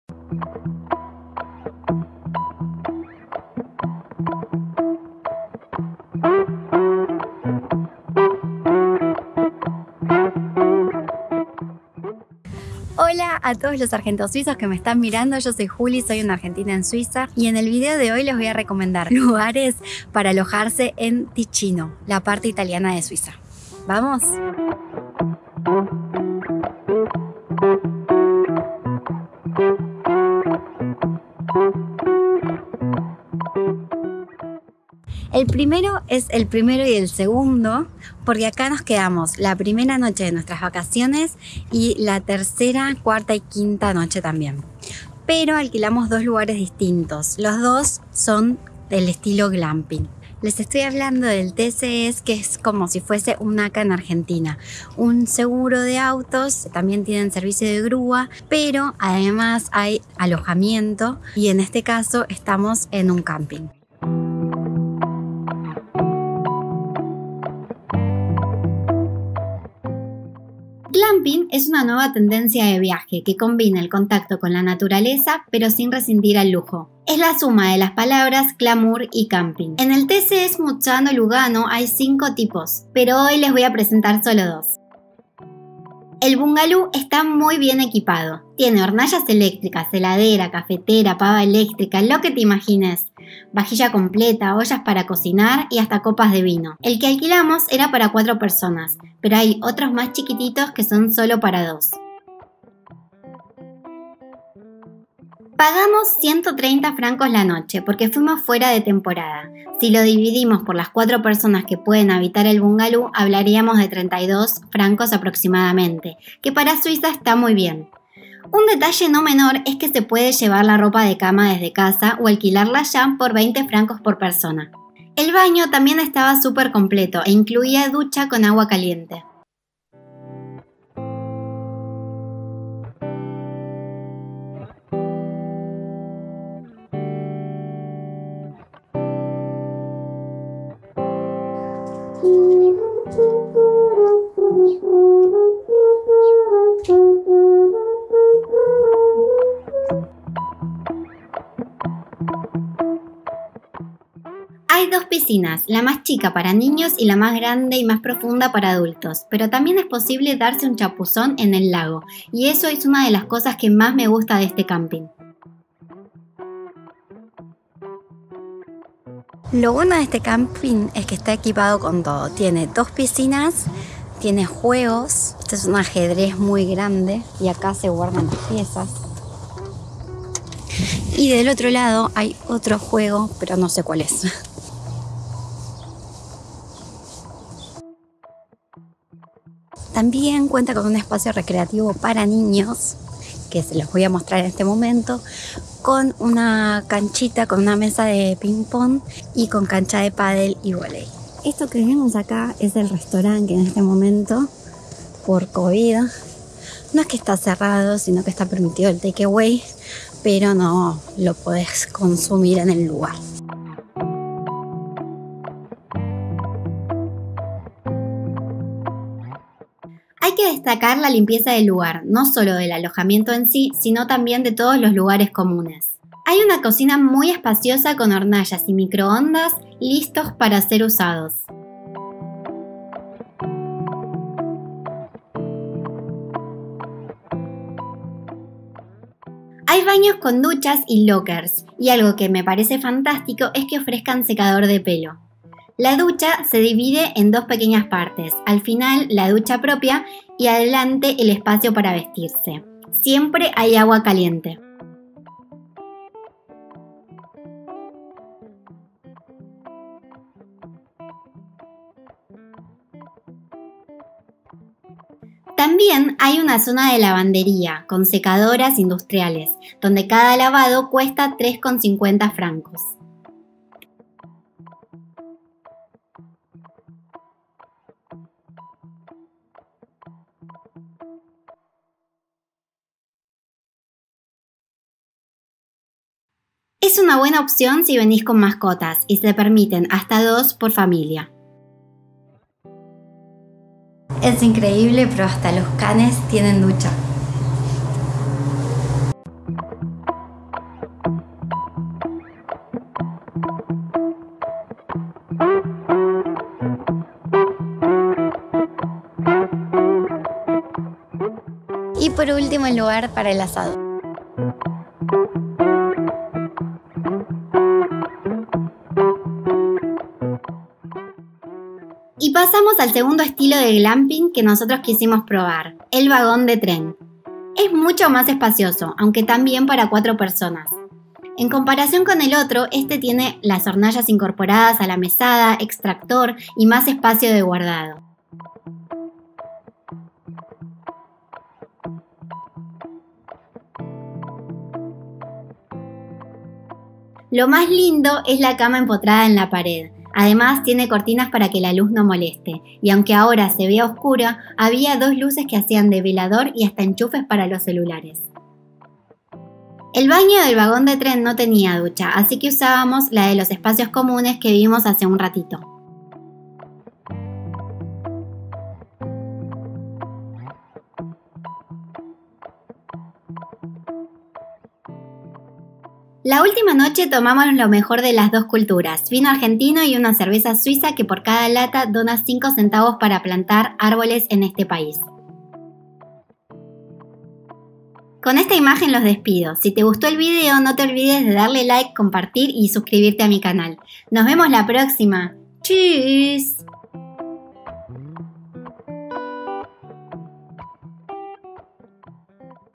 ⛺ Cómo es un CAMPING en SUIZA 🇨🇭 Cuánto cuesta y qué es hacer GLAMPING? 🏕️ Turismo en Lugano
Les muestro cómo es un camping en Suiza y qué opciones de Glamping existen.

✨Glamour + ⛺️Camping = GLAMPING 😏

🤩 Es un término relativamente nuevo y hace ya un tiempo me moría por probar algo así. Hace referencia a un tipo de turismo sostenible 🌲🌳🌴 donde el alojamiento conecta con la naturaleza por estar al aire libre pero sin dejar de lado las comodidades de un hotel.

🎥¿Querían saber cómo era por dentro? Acá se les muestro!

Los precios serán los mismos, pero me ayudarás a recibir una comisión por tu compra.